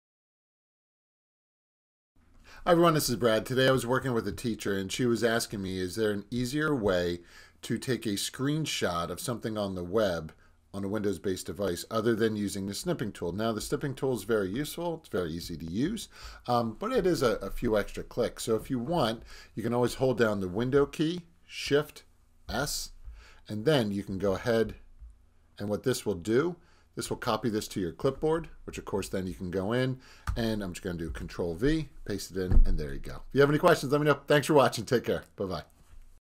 How to take a quick screenshot on your Windows based device - Windows Key + Shift + s
Today I was working with a teacher who mentioned how she would love if there was a shortcut to take a snip of an image on her screen (other than using the Snipping Tool).

Well, there is. Simply use the (Windows Key) + (Shift) + (S).